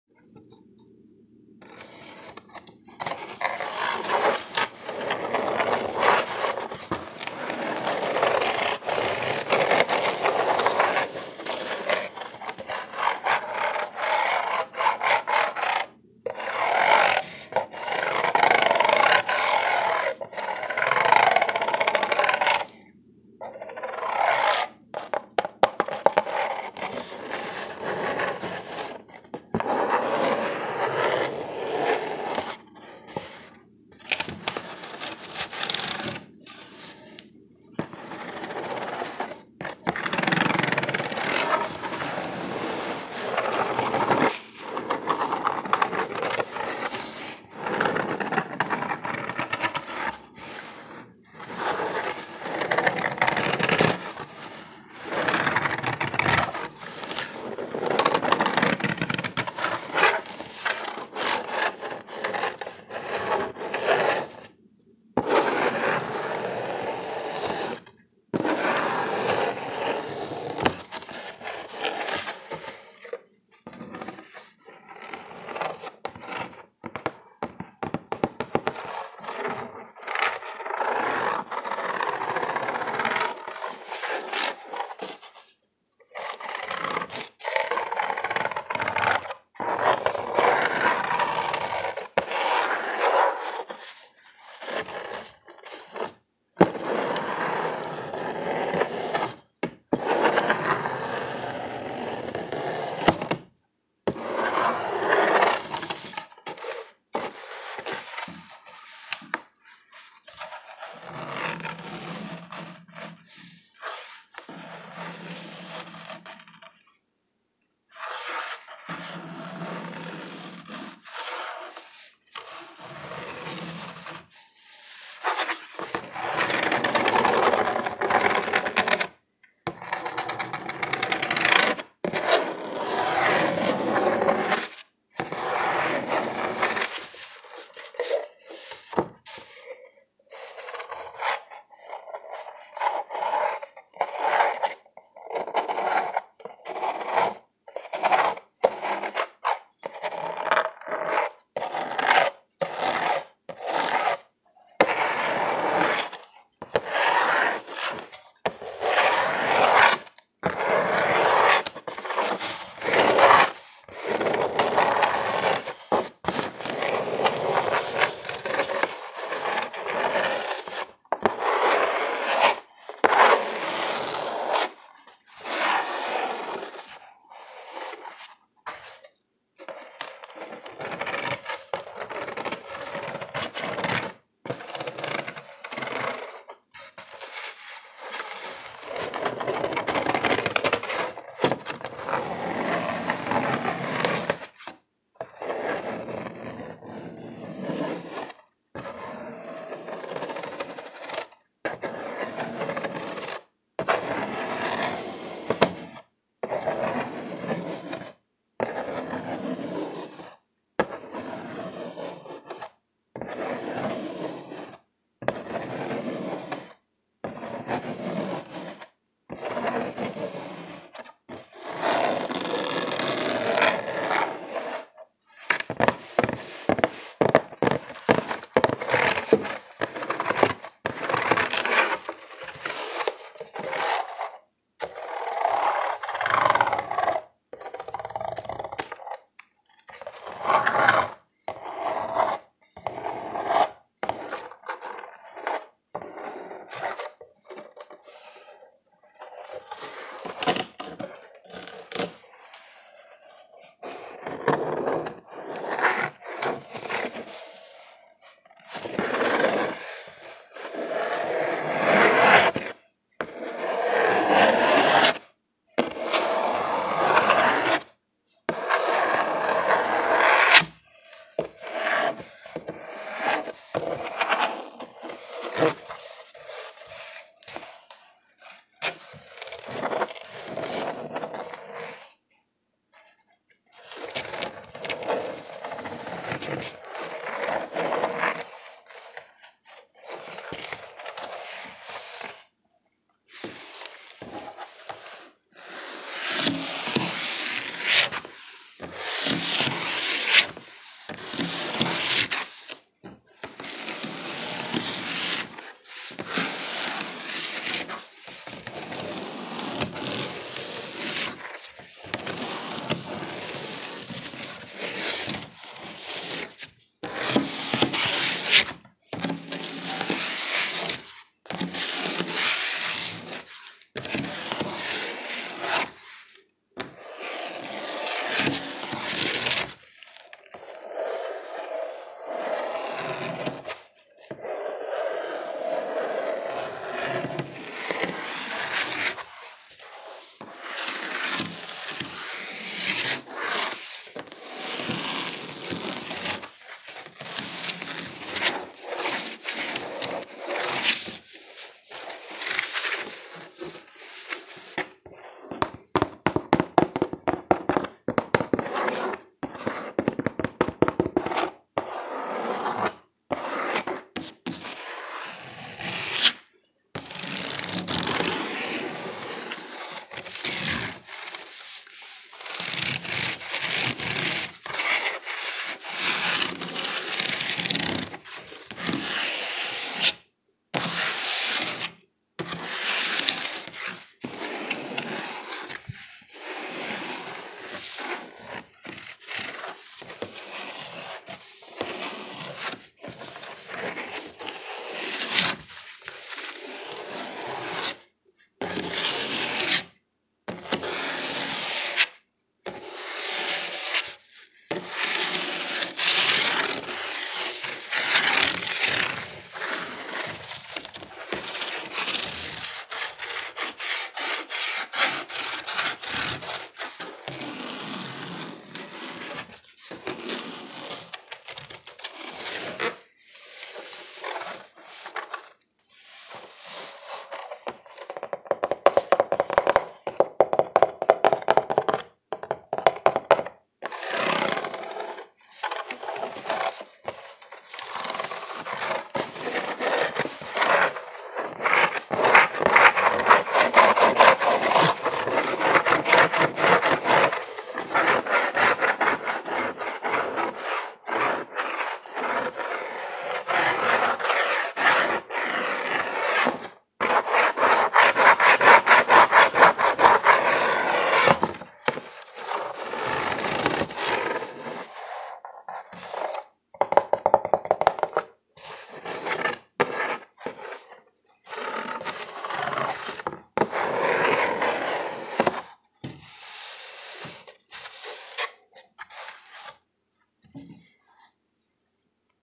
ASMR Hard Scratching and Tapping
Hi guys! I'm back!

In this video there will be hard scratching and a little tapping on some cardboard! Some people enjoy hard sounds so for those of you who do, please enjoy!

If you do not like hard tapping or scratching, do not watch the video!